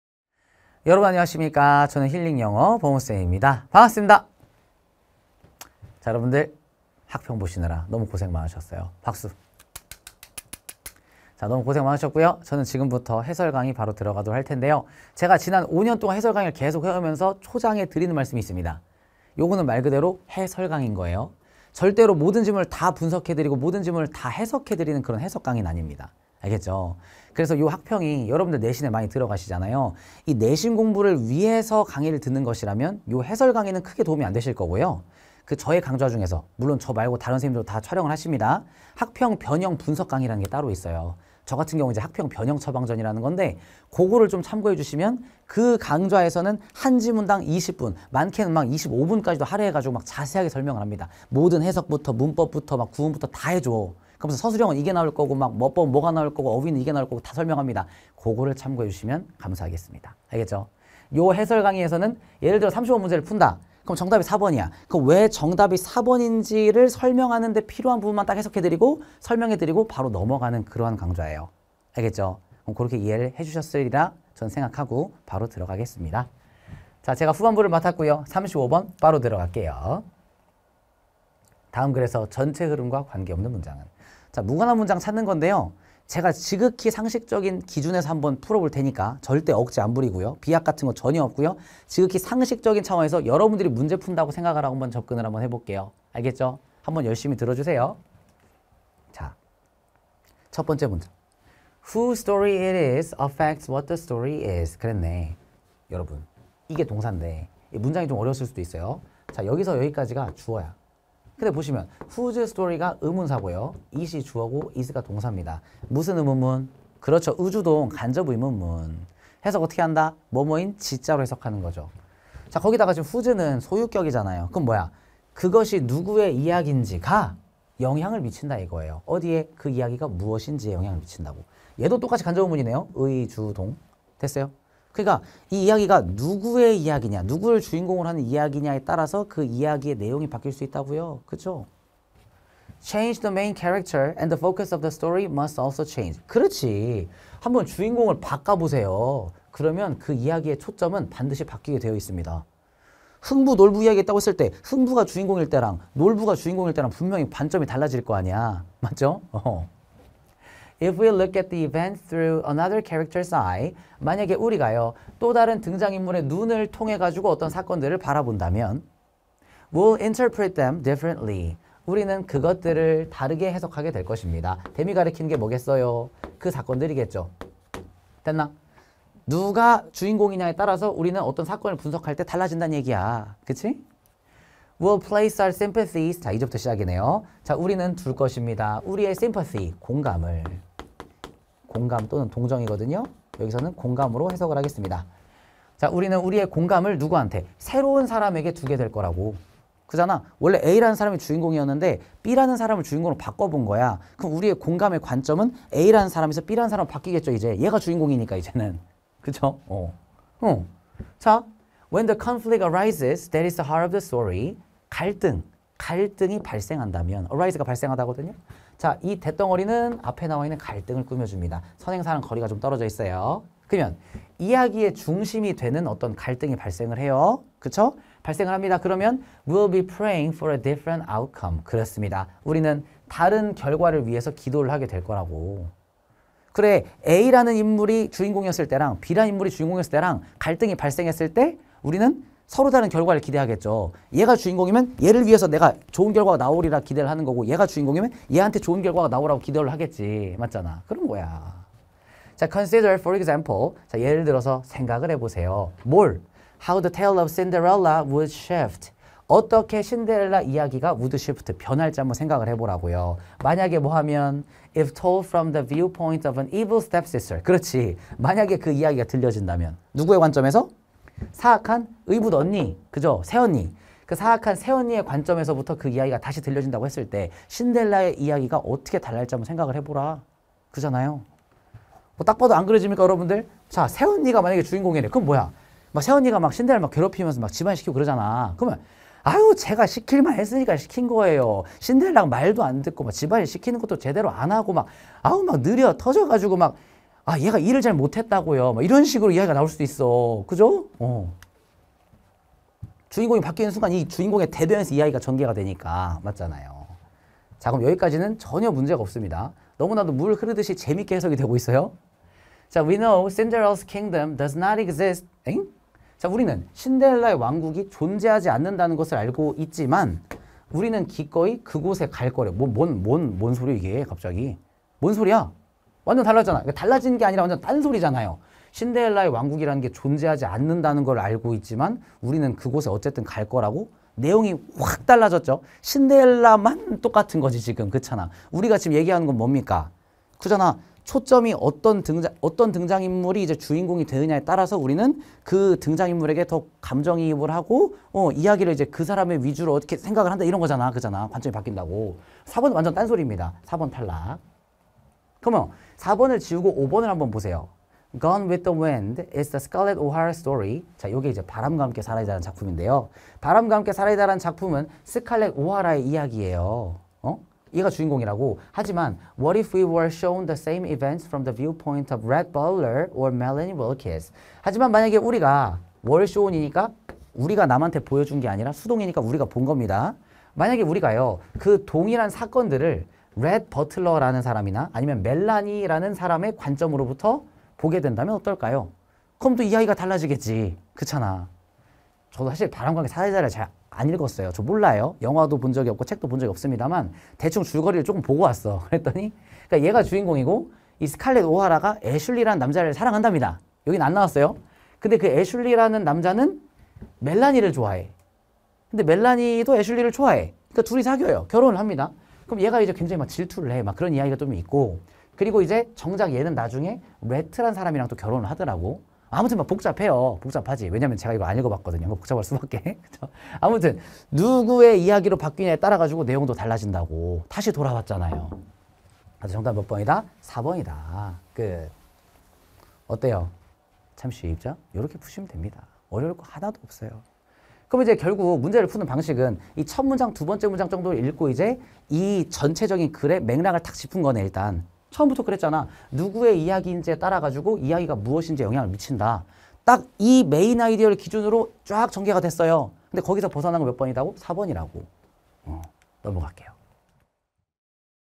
[메가스터디] 영어 김범우쌤 - 2023년 고1 3월 학평 영어 해설강의
영어 김범우 선생님의 2023년 고1 3월 학평 영어 해설강의 영상입니다.